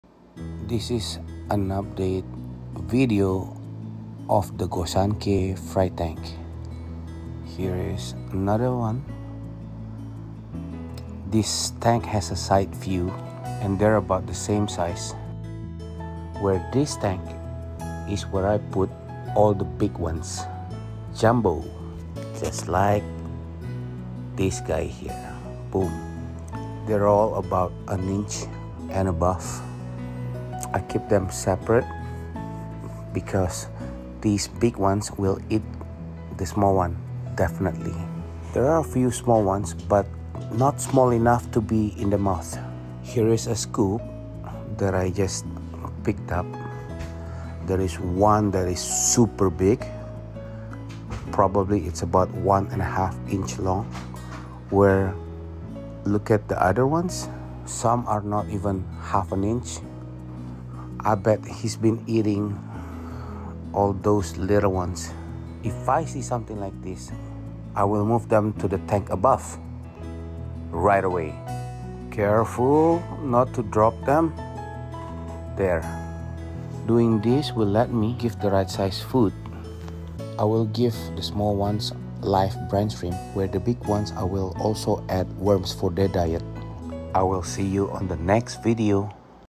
Growth rate on 3-4 weeks old Koi fry
Hey, I'm Dede and you can call me D.
This is my first experience breeding koi. These are the hatchlings from the Gosanke mass spawning from June.
I'm new to koi keeping and I hope this process video would help at least anyone new too.
And I hope I can learn from the experience and expert koi keeper.
Check my current state of koi keeping and other fish on my Instagram/Facebook at BigDvsFish